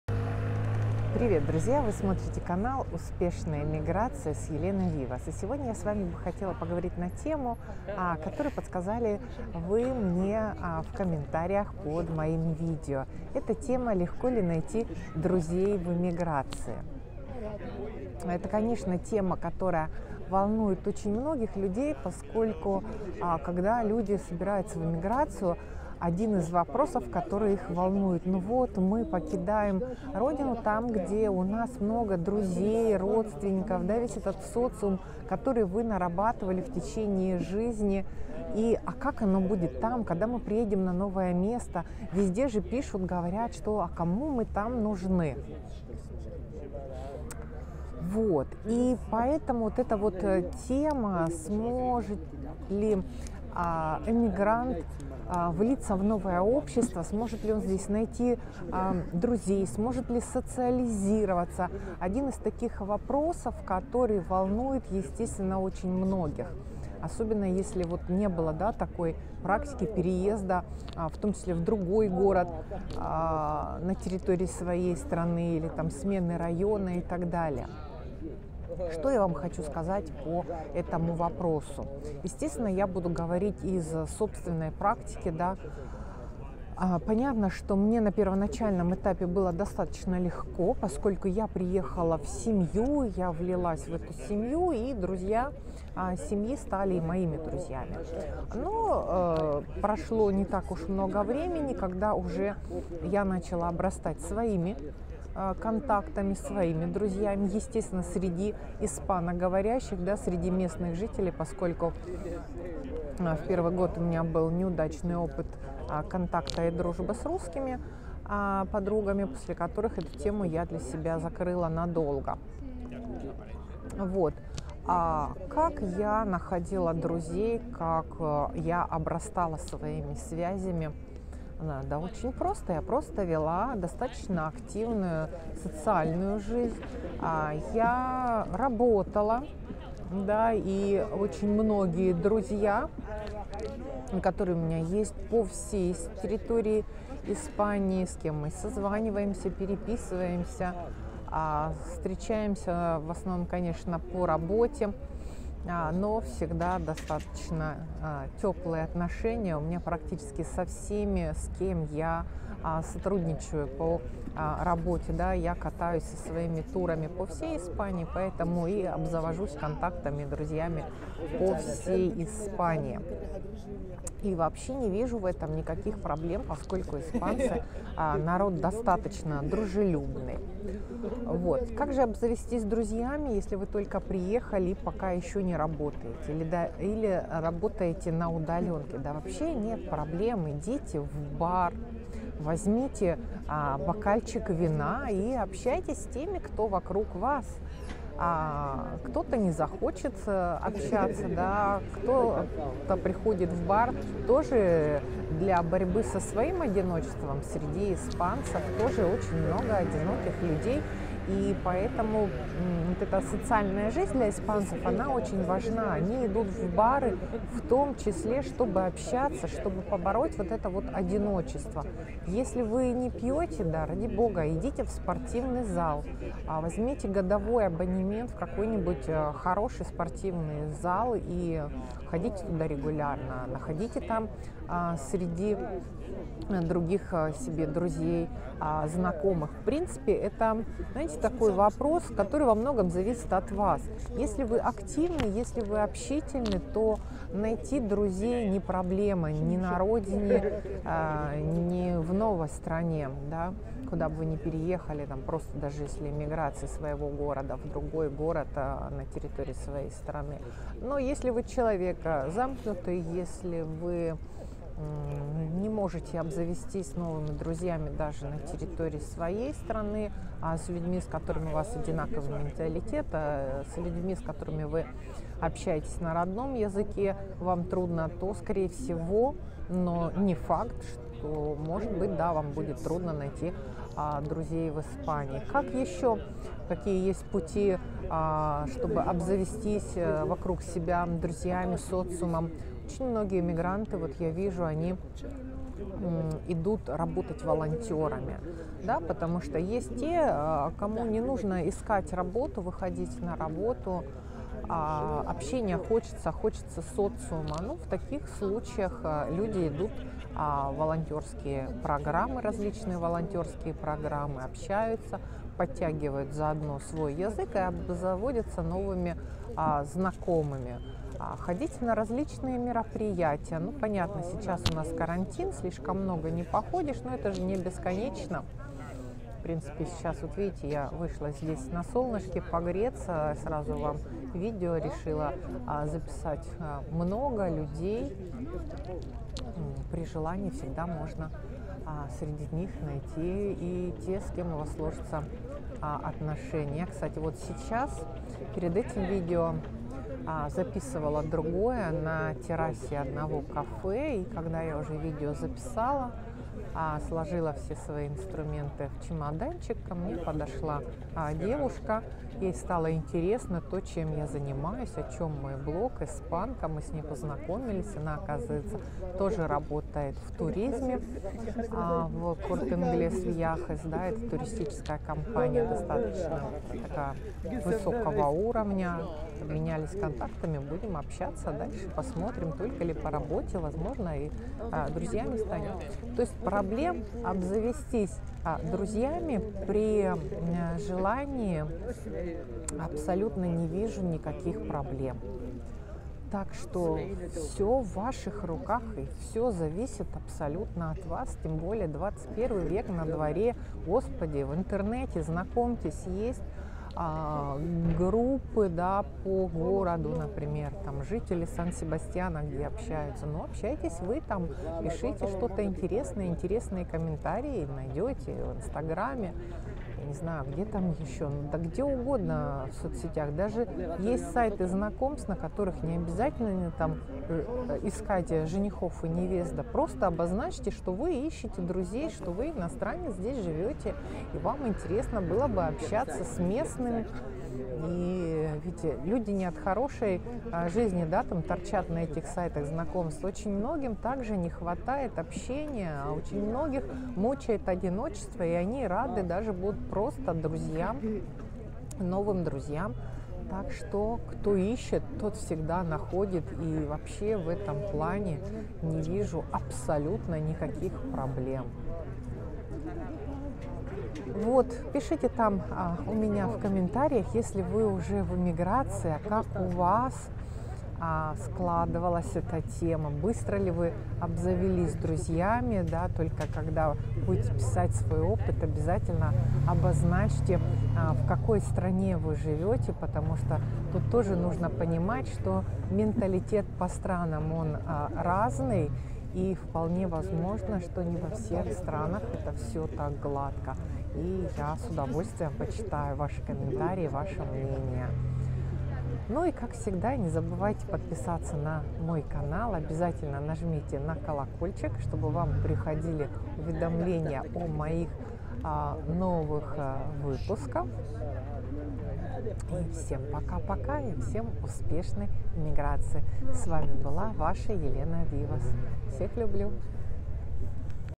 Трудно ли найти друзей в эмиграции? Жизнь в Испании
Легко ли найти друзей в эмиграции? Ответ очевиден.